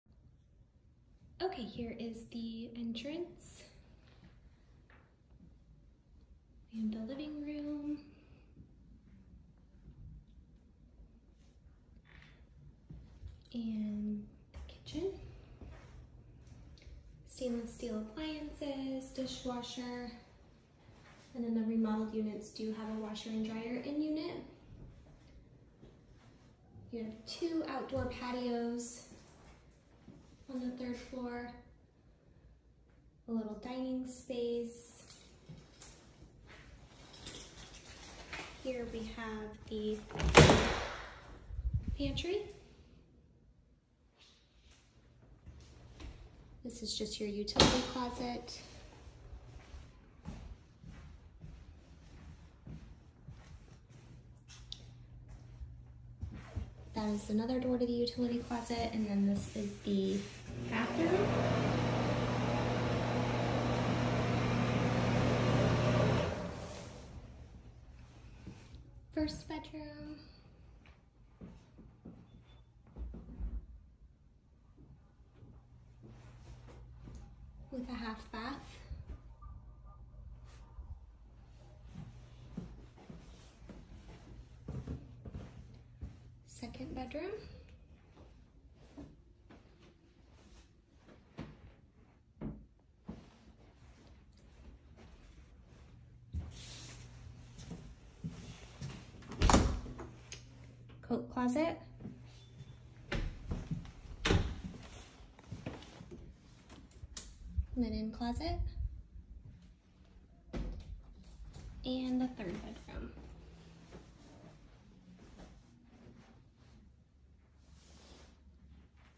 Lincolnshire West -3 bedroom 1.5 Apartment;
Remodeled unit
Available now!! This spacious 3 bedroom - 1.5 bath apartment has just undergone a complete renovation. This gorgeous home features an all new kitchen with new cabinets, countertops, and appliances (including microwave and dishwasher). You'll find all new flooring throughout, new paint, doors, and trim. In-unit washer and dryer included! This community is centrally located near MTD bus stops, schools and shopping!
We are also pet friendly with no breed or size restrictions.
$84 monthly utility fee includes water, sewer, trash, lawncare, snow removal and parking